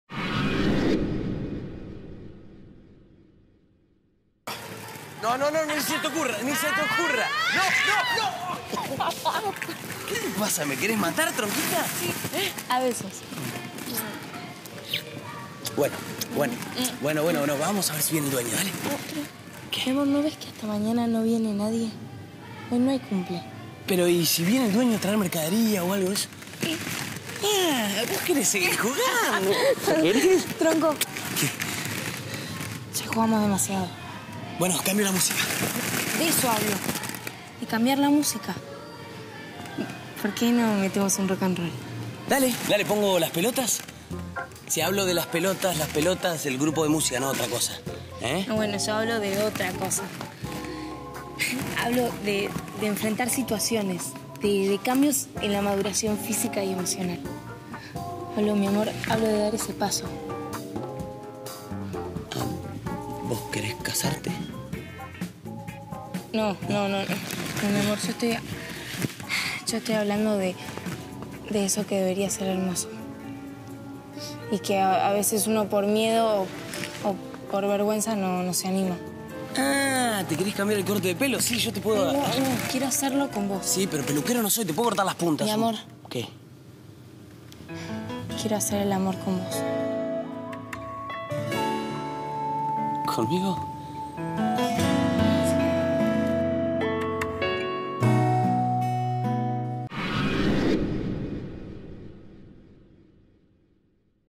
VALE Y RAMA EN EL PELOTERO PARTE IV
Vale lleva a Rama al lugar de juegos donde iba a jugar de chica.